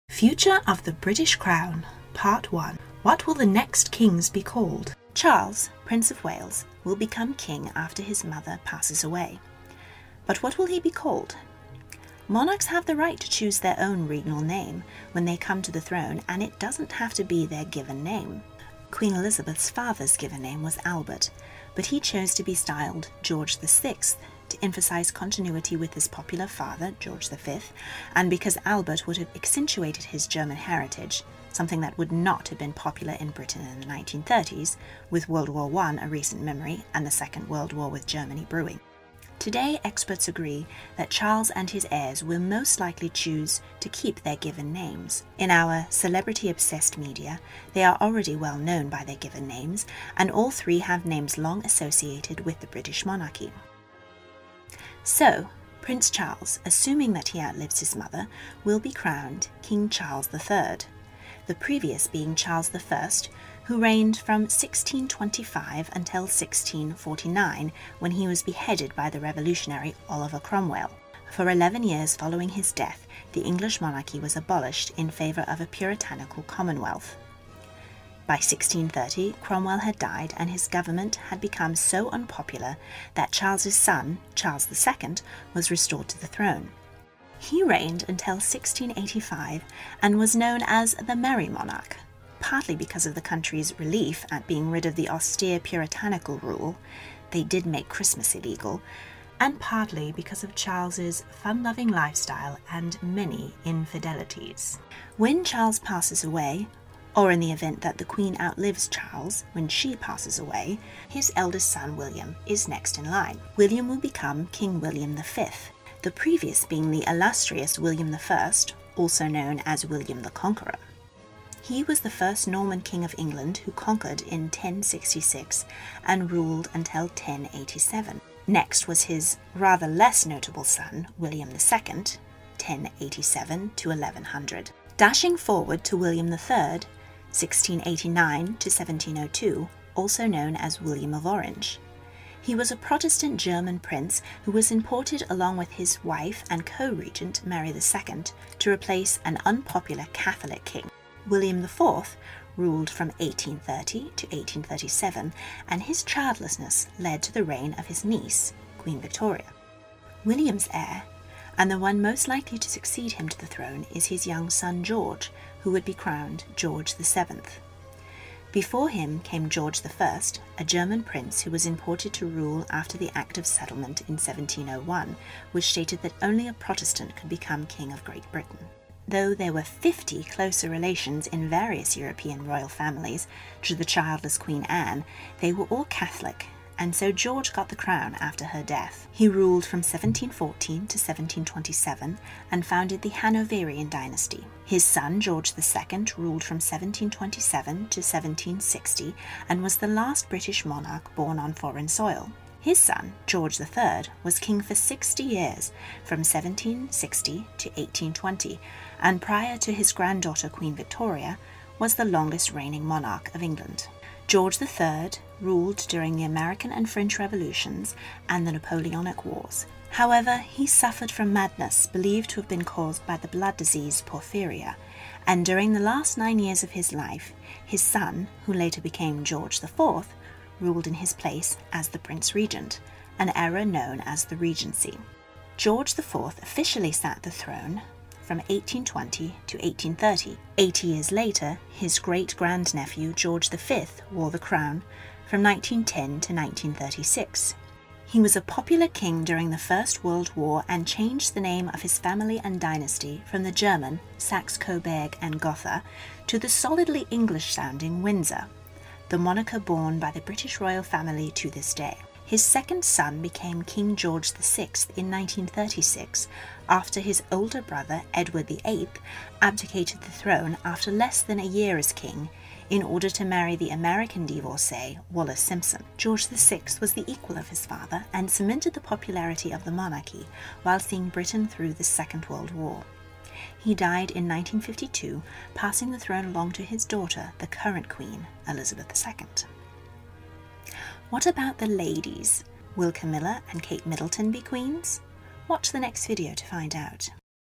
Future of the British Crown 1/4: What will the Next Kings be Called?
and help me make more fascinating videos!

King Charles III, King William V, King George VII. History of the previous kings of those names.

Explanation of the likely future of the British monarchy with historical references. Part one of four: What will the Next Kings be Called?